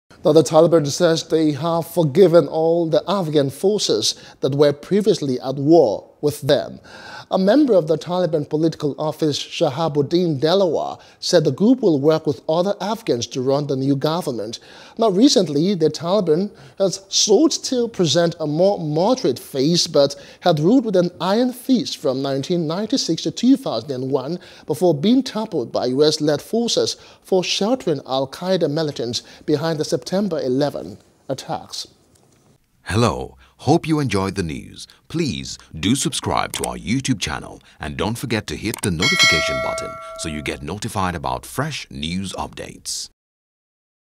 Afghanistan: 'We Forgive Our Enemies' - Taliban Official
The Taliban says they have forgiven all the Afghan forces that were previously at war with them.